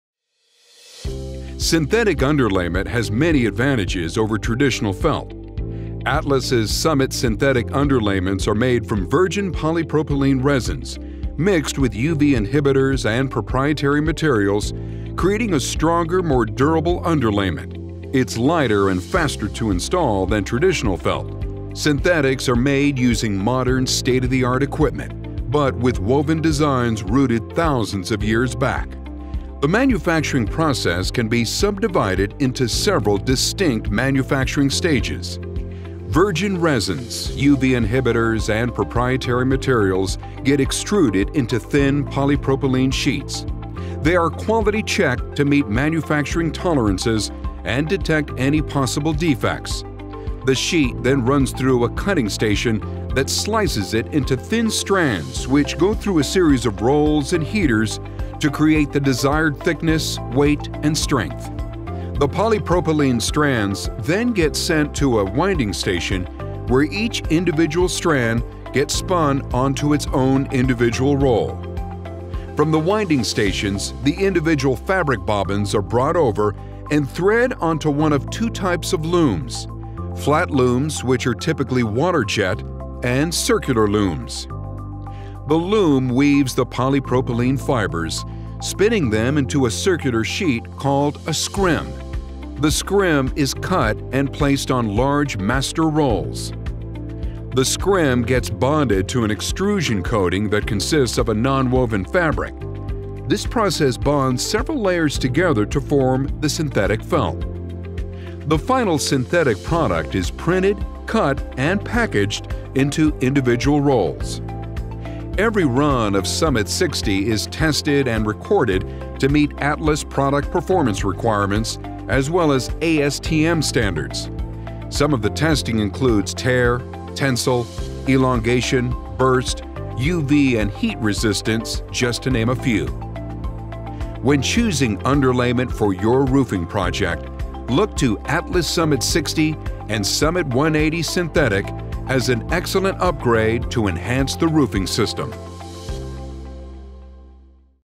How It's Made: Atlas Summit Synthetic Roofing Underlayment
This video shows the process used to manufacture Atlas synthetic underlayment, used in all types of steep slope roofing systems. These 4-layer products have unique characteristics to enhance roof performance and provide benefits to installers.